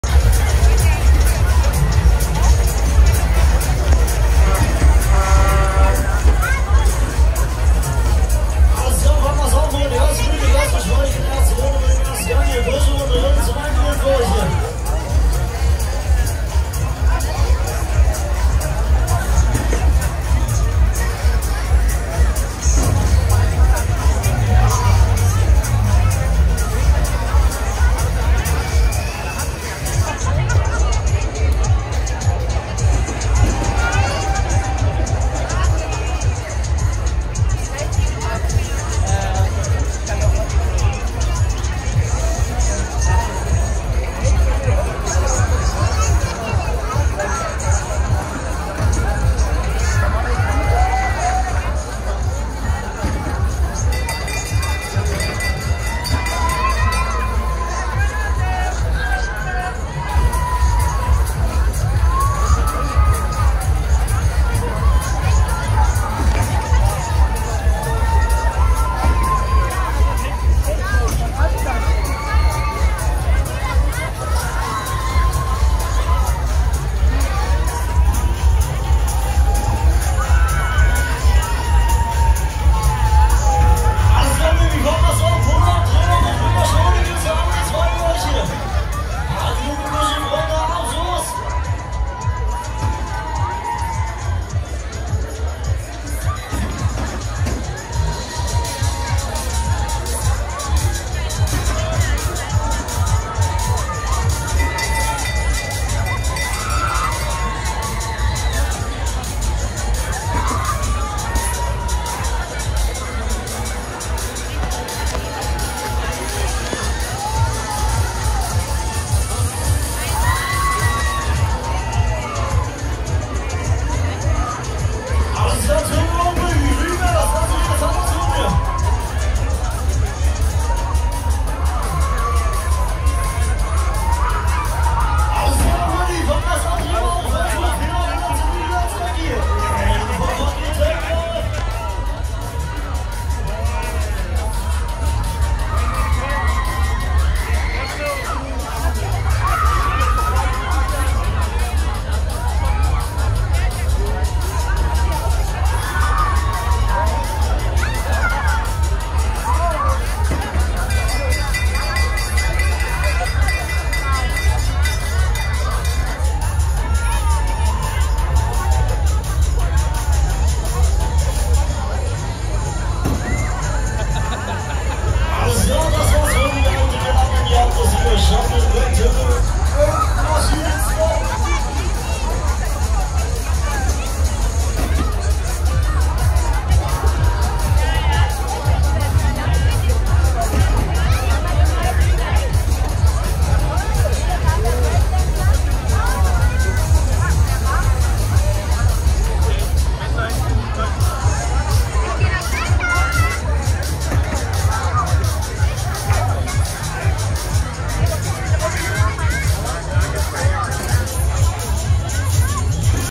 ⁴ᴷ Street Fighter - Jacobi - Offride | Burgfest Tangermünde 2023
Street Fighter von Jacobi Offride Video auf dem Tangermünder Burgfest 2023. 20 Meter Technical Park Action Schaukel Street Fighter Kirmes Volksfest Rummel Burgfest Tangermünde 2023.

Schausteller: Jacobi
Hersteller: Technical Park (Italien)
Typ: Street Fighter
Baujahr: 2004
Fahrpreis: 5,00€